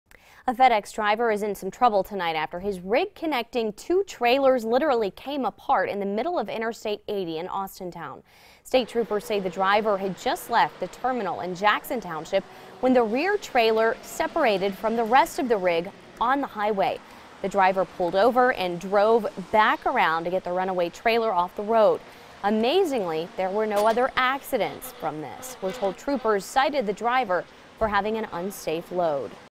FedEx trailer detaches from rig, comes to stop in middle of I-80
The driver is in some trouble after his double-trailer rig came apart in Austintown.